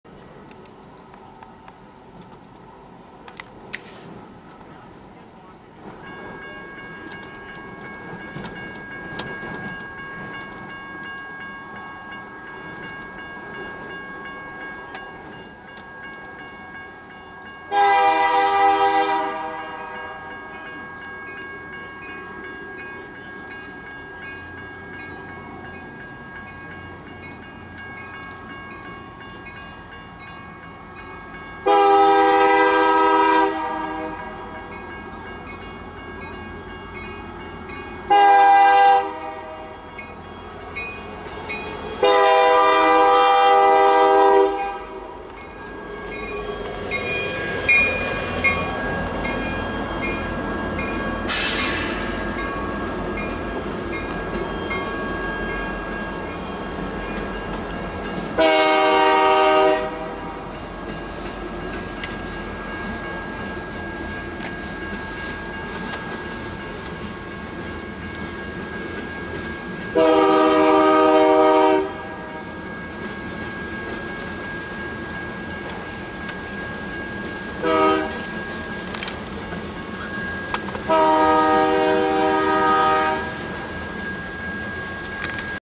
UP 4079 North Bellevue, OH
NB Manifest, Monroe St., Bellevue, OH
UP 4079 = SD70M - Wings - Old K3H
NS 9045 = C44-9W - Old NS
NS 6956 = SD60E - NS HH